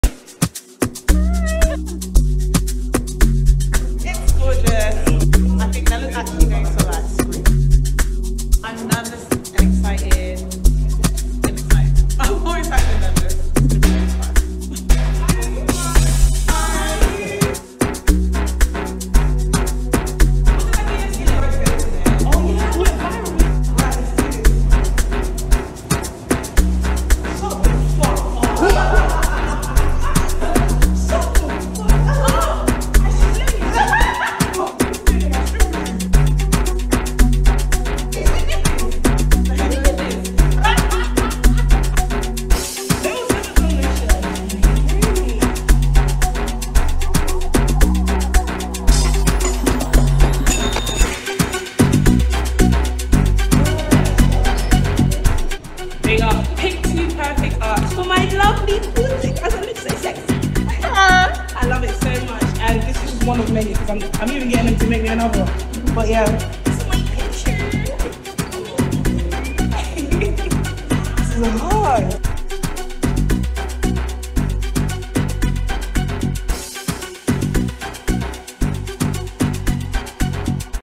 Pic2Perfect Art gifts Nella Rose a painting
Creative direction, filmed and edited by Mount Social for Client